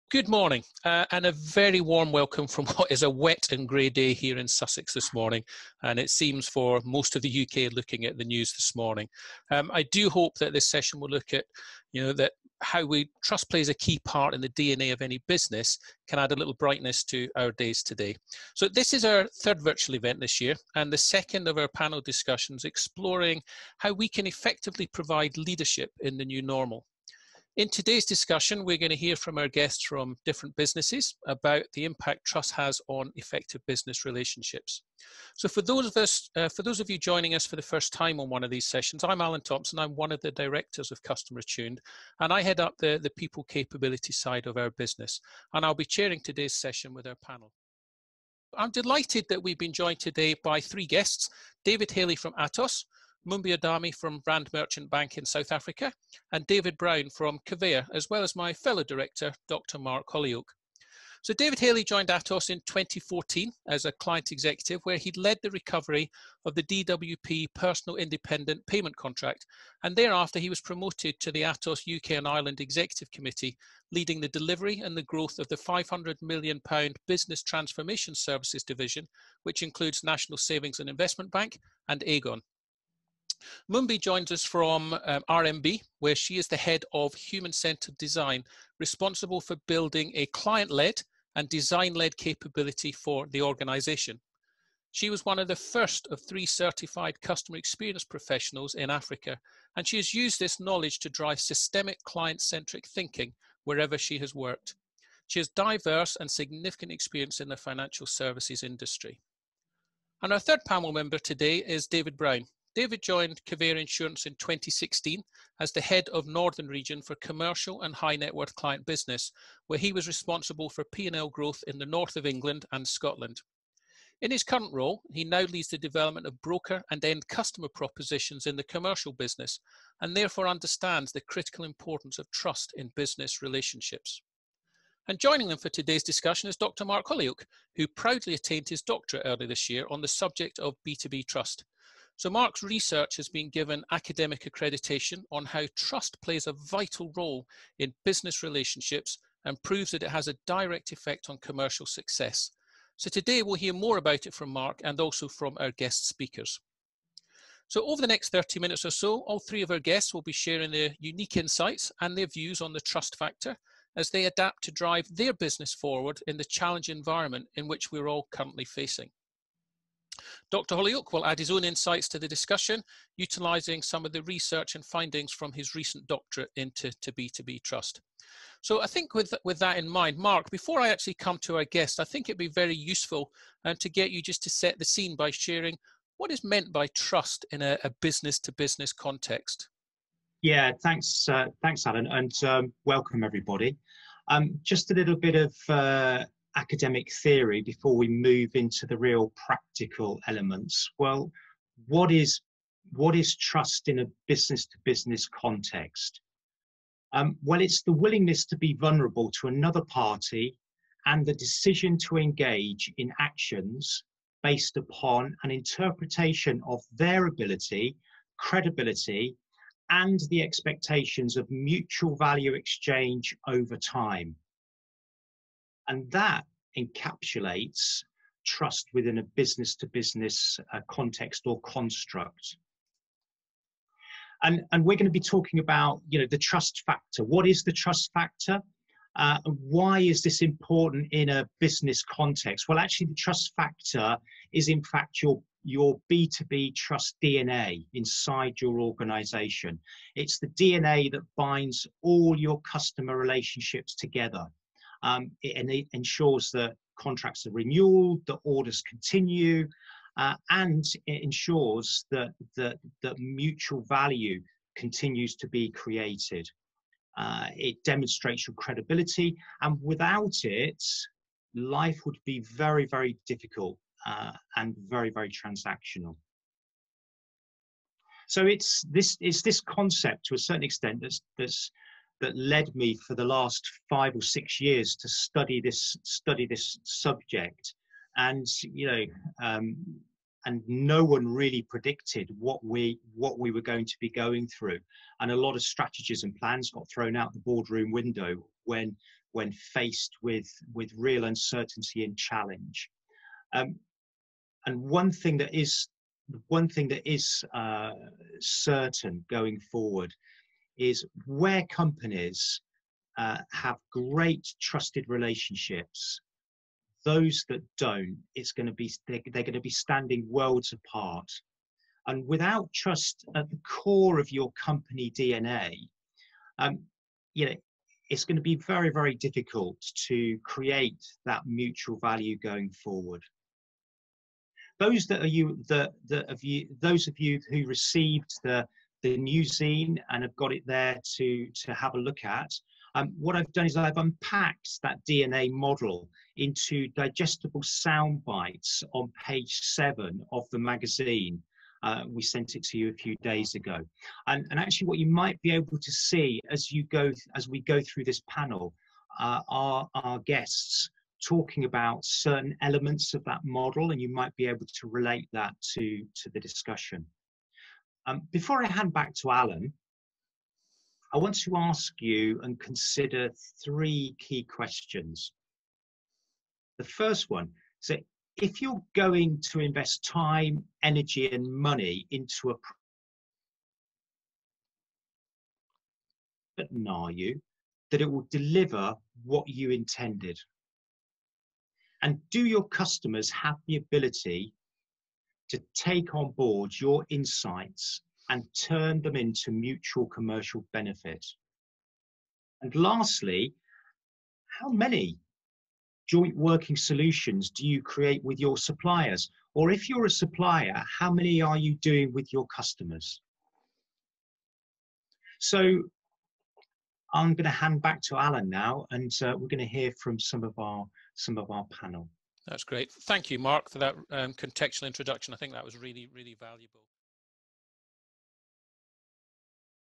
The Trust Factor Panel Discussion - Introduction to B2B Trust
Welcome to the third online panel discussion and second in the series of Providing Leadership in the New Normal.

In this panel discussion, we discuss The Trust Factor why making it your business DNA is crucial especially during the COVID pandemic.

On this panel, we have leaders from across sectors sharing their insights with us:
- David Haley - Head of Business Transformation Services, Atos
- Mumbi Odame - Head of Human Centred Design, Rand Merchant Bank
- David Brown - Distribution Proposition & Agency Leader, Commercial, Covéa Insurance

Together with Dr Mark Hollyoake, who explains why B2B Trust is a crucial factor in business to business relationships.

In part one, Mark explains the theory behind B2B Trust.